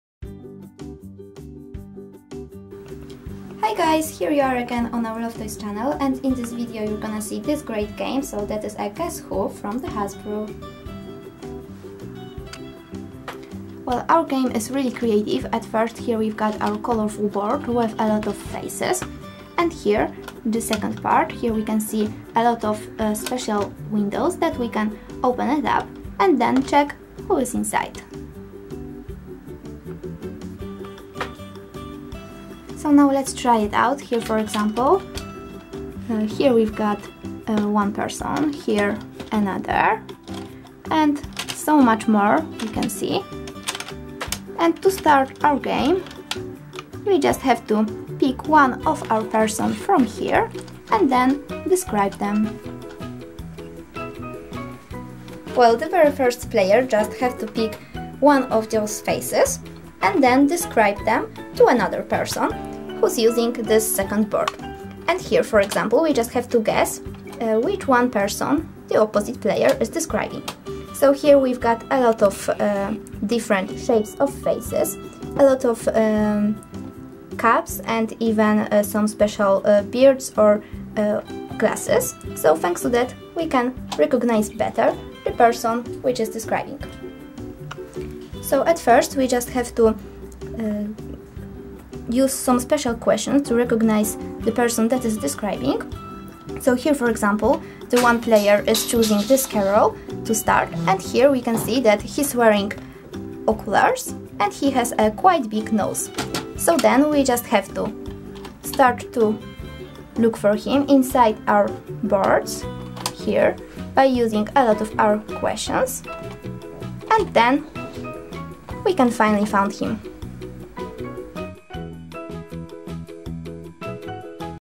Hasbro Games - Guess Who? - 05801
Play the original mystery face-guessing game and have hours of guessing fun!  Eliminate the wrong faces to guess who's on your opponent's card!  Two players.  Ages 6 and up.